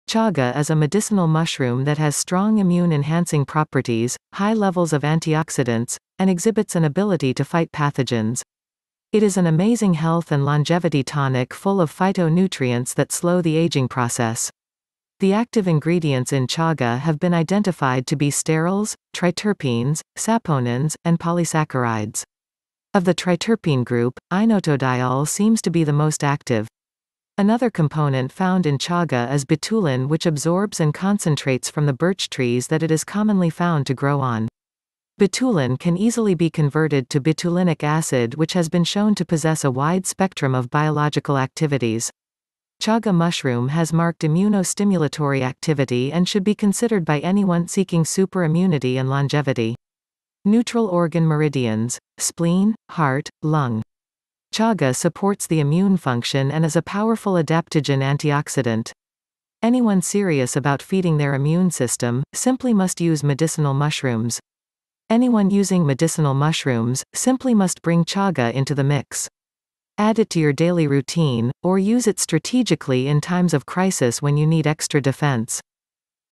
Jing Herbs Chaga - Immune Powerhouse. Antioxidant Rich. Longevity Herb. Polysacharide Rich.
For USA orders:

Chaga is a medicinal mushroom that has strong immune enhancing properties, high levels of antioxidants, and exhibits an ability to fight pathogens.

It is an amazing health and longevity tonic full of phyto-nutrients that slow the aging process.

The active ingredients in Chaga have been identified to be sterols, triterpenes, saponins, and polysaccharides.

Of the triterpene group, inotodiol seems to be the most active. Another component found in Chaga is Betulin which absorbs and concentrates from the birch trees that it is commonly found to grow on.

Betulin can easily be converted to Betulinic acid which has been shown to possess a wide spectrum of biological activities.

Chaga mushroom has marked immuno-stimulatory activity and should be considered by anyone seeking super immunity and longevity.

neutral
Organ Meridians: Spleen, Heart, Lung.
Chaga
supports the immune function
and is a powerful adaptogen
antioxidant. Anyone serious about feeding their immune system, simply must use medicinal mushrooms. Anyone using medicinal mushrooms, simply must bring Chaga into the mix. Add it to your daily routine, or use it strategically in times of crisis when you need extra defense.